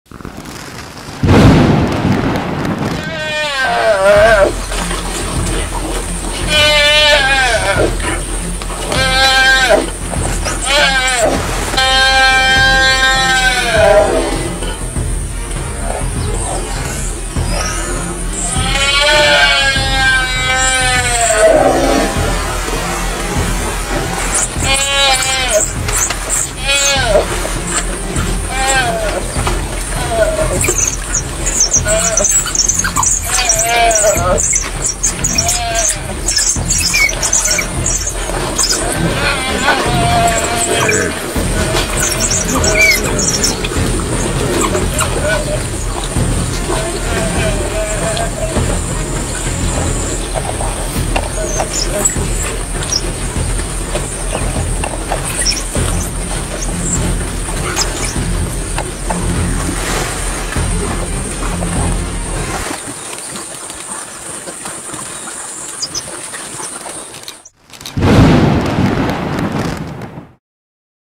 (Nature) Wild dogs Vs young deer 🦌
wild dogs eat a deer nature